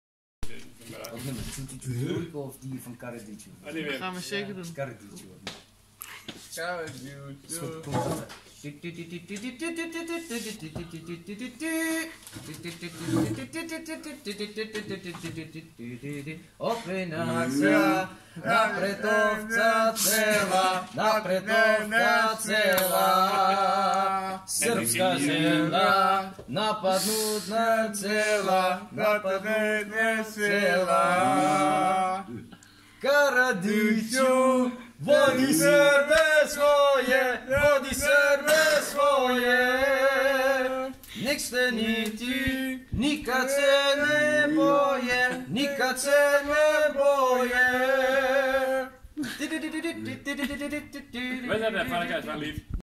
Months ago singing with my Dutch friend Serbia is strong!
On my work. 🇷🇸💪💗